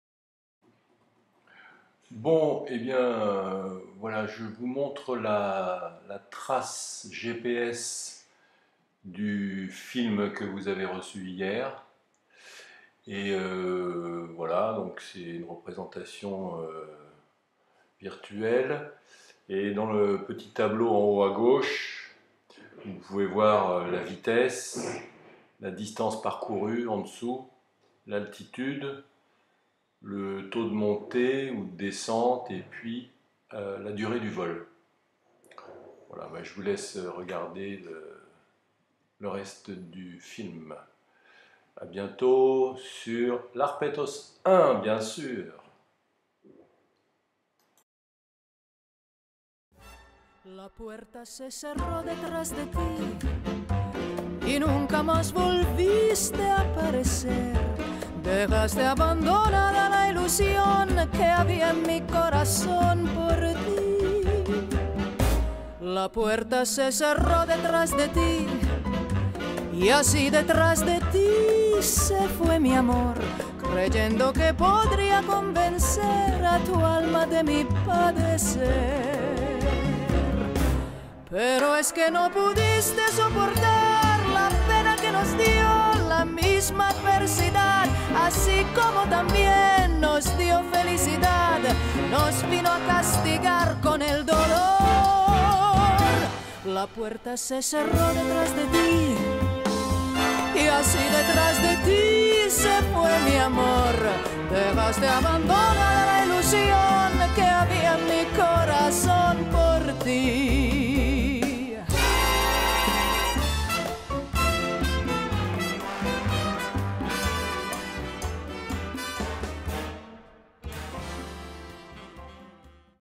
trace du vol d'Annecy
Distance parcourue 27 km en 55 mn
altitude max. 1 800 m
Voile NIVIUK Hook 3